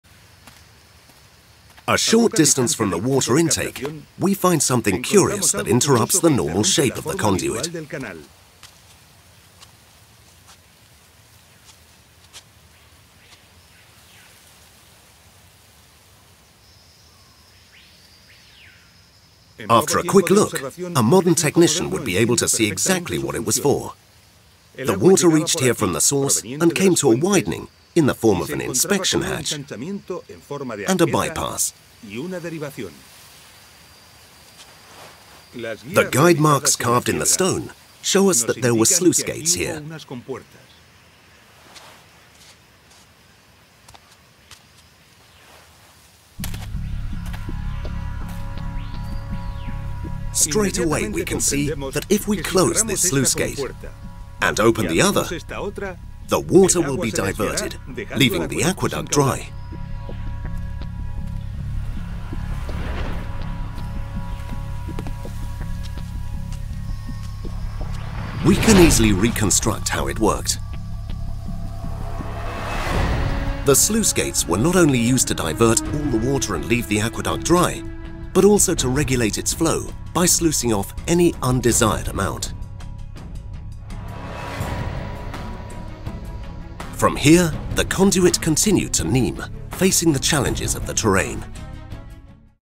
Mastering Water Flow: Roman Aqueduct Gate Systems
Dive into the ingenious water management techniques of the ancient Romans as we unveil the secrets of controlling water flow in aqueducts. Discover how Roman engineers utilized sophisticated gate systems to regulate the flow of water, allowing them to divert streams as needed for maintenance or adjust water levels to meet varying demands. Join us on an exploration of hydraulic mastery as we uncover the intricacies of these gate mechanisms and their pivotal role in sustaining the lifeline of ancient cities. Learn about the specific gate system employed in the aqueduct that carried water from Uzès to Nemausus (Nîmes), France, and marvel at the remarkable ingenuity that powered the aqueducts of ancient Rome.